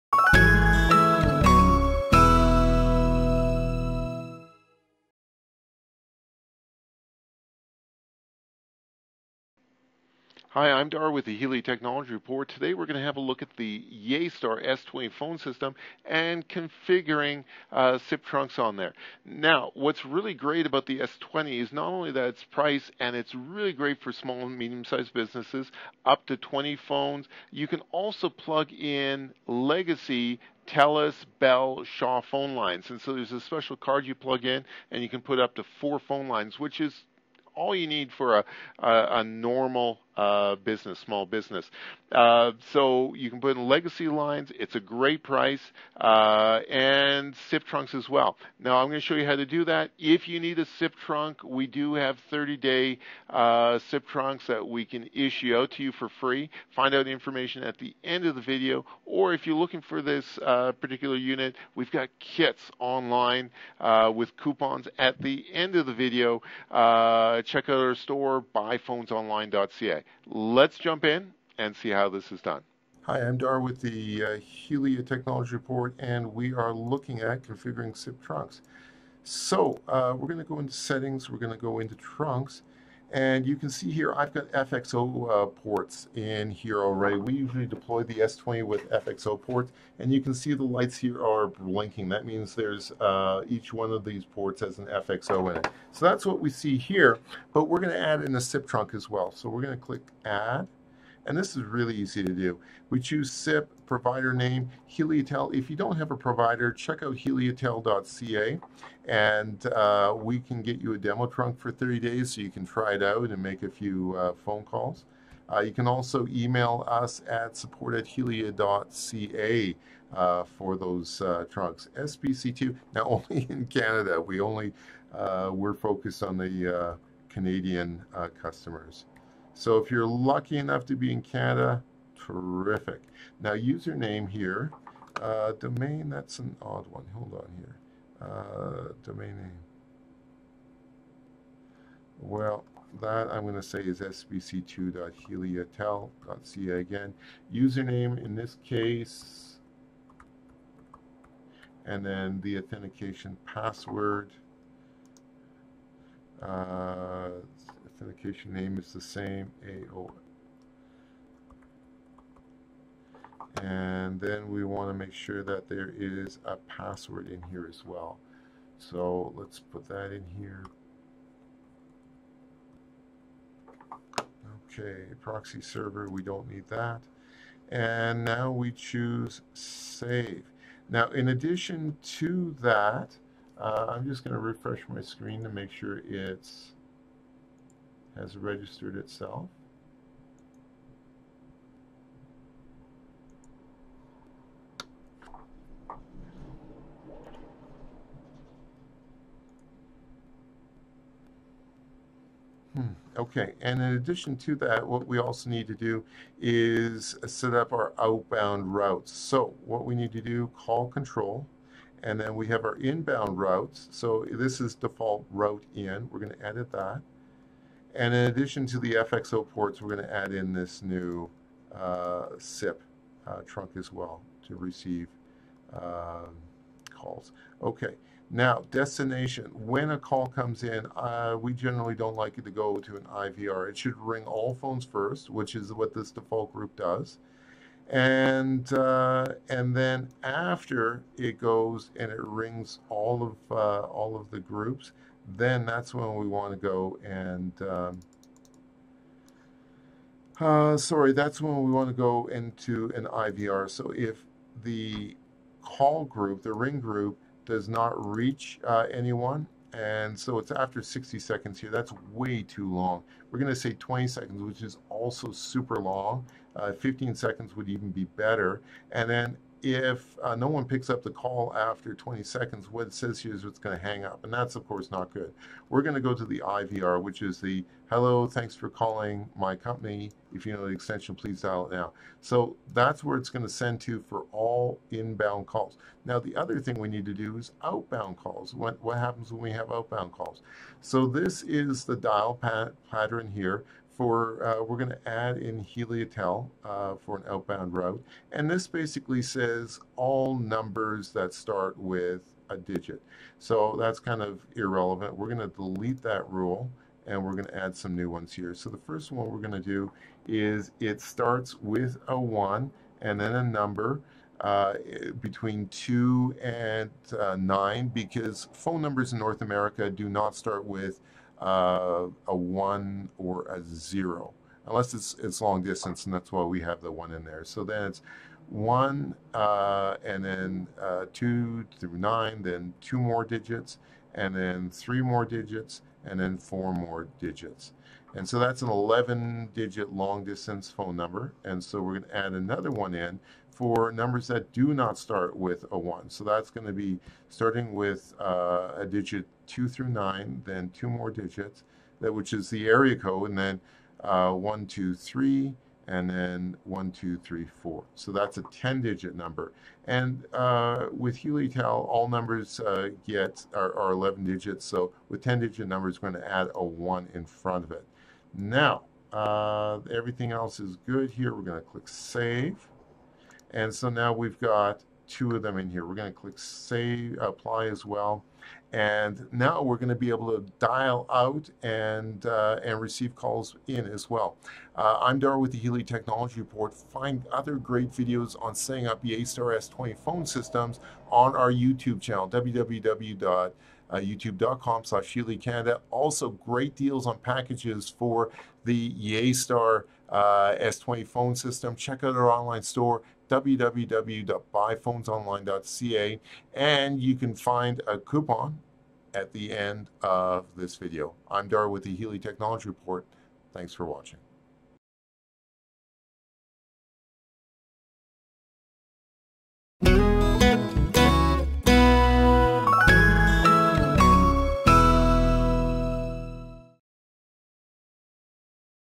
How To Configure SIP Phone Lines on the Yeastar S20
The Yeastar S20 Phone System is great for Small business.  It accepts legacy Telus, Shaw, and Bell phone lines and is very well priced.  Find out how to configure SIP lines on your Yeastar S20.